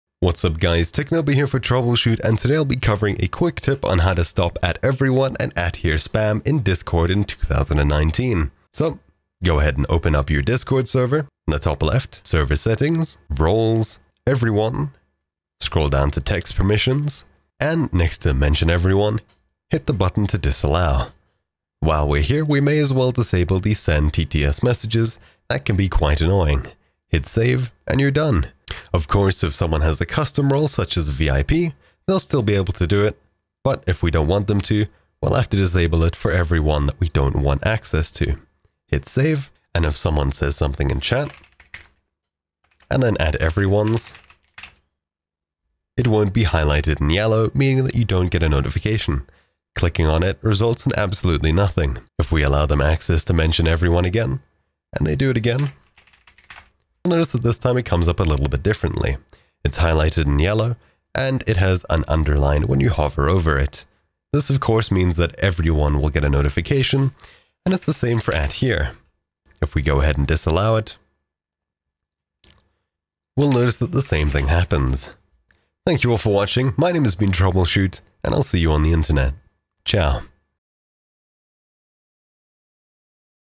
STOP @everyone AND @here SPAM! (Discord \\ Full Guide) - How To
If you're a server owner, and you're looking at stopping the relentless "boops" of @everyone and @here spam,  then here's the solution!

Timestamps
0:00 - Introduction
0:12 - Server settings
0:40 -  Demo